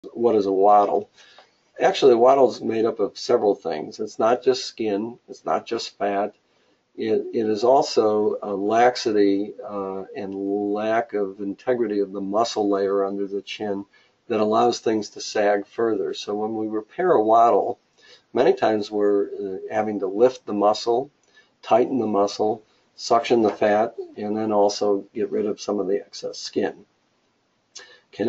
What is a Wattle?
Dr. Kaplan answers your questions on Beautiful Silhouettes: Noses, Chins, Ears & Necks Facts.